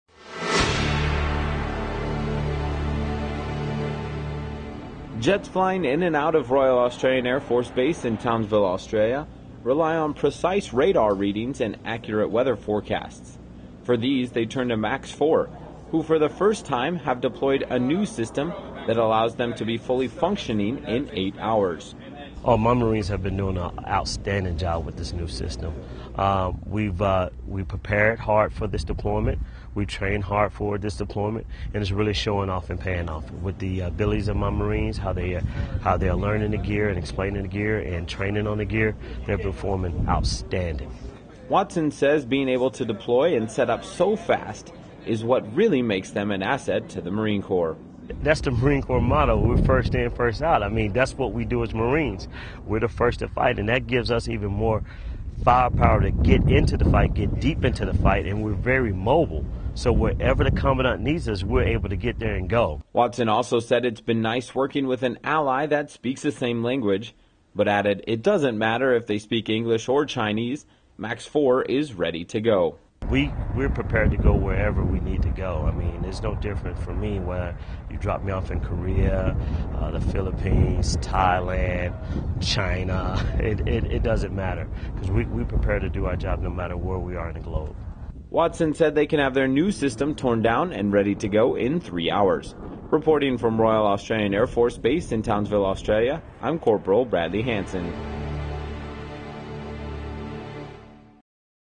Marine Air Control Squadron 4 deploys new system for first time at Talisman Sabre 2011
Anywhere Marine Corps jets are flying, there's a good chance you'll see a Marine Air Control Squadron with them.  Cpl. Bradley Hanson caught up with MACS-4 in Townsville, Australia.